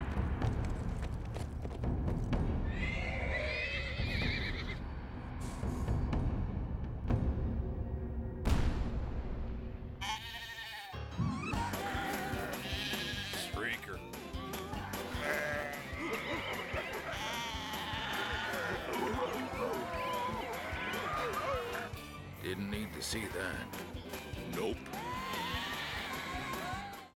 Budweiser Streaker Commercial - High Definition
High Definition Budweiser Commercial from the 2006 Superbowl featuring those famous Clydesdale  horses.